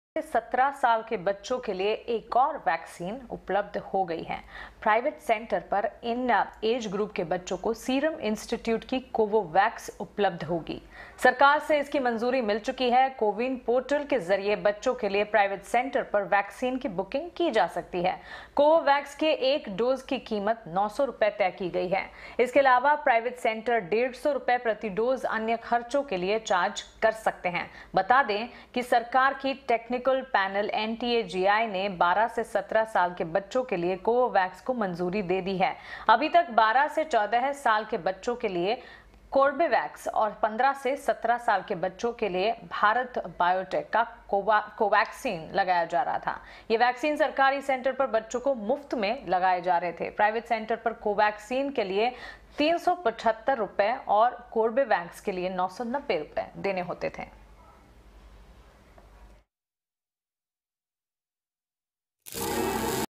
Serum Institute के Covovax को NTAGI से मंजूरी, 12 से 17 साल वालों को लगेगा टिका | Swadesh Khabar
सीरम इंस्टीट्यूट के Covovax को NTAGI से मंजूरी, प्राइवेट सेंटर पर उपलब्ध, CoWIN पोर्टल के जरिए होगी Covovax की बुकिंग